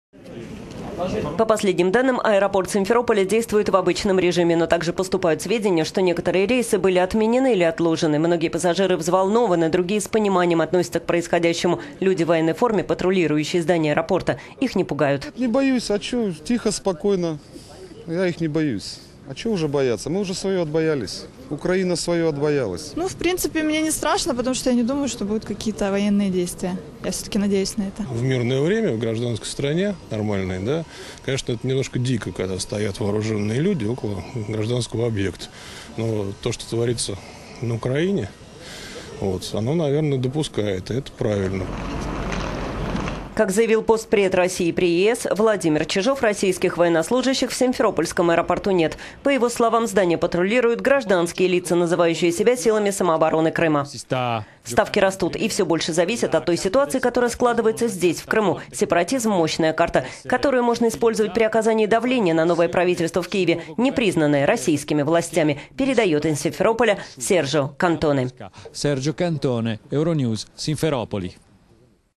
Симферополь. Пассажиры не боятся вооруженных людей у аэропорта
По последним данным, аэропорт Симферополя действует в обычном режиме. Но также поступают сведения, что некоторые рейсы были отменены или отложены. Многие пассажиры взволнованы, другие с пониманием относятся к происходящему - люди в военной форме, патрулирующие здание аэропорта, их не пугают.

- Нет, я не боюсь. А что? Тихо, спокойно. Чего их бояться? Мы свое уже отбоялись. Украина свое отбоялась.

- Ну, в принципе, мне не страшно, так как я не думаю, что будут какие-либо военные действия. Я ...